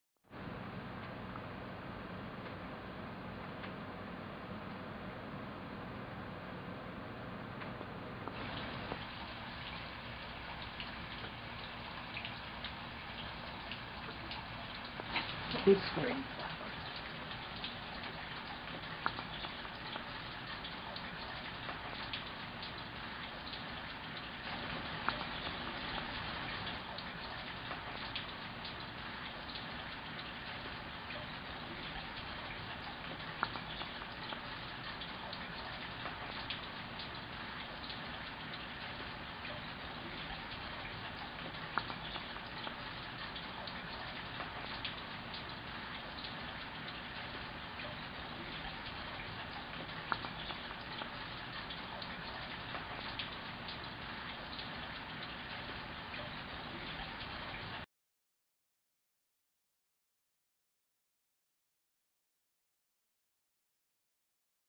Early Spring Snow in Eugene, Oregon
A wet snow began to fall at 4:00 in the afternoon on Friday March 23rd in Eugene, Oregon. The snow was pretty to watch, but it didn't stick.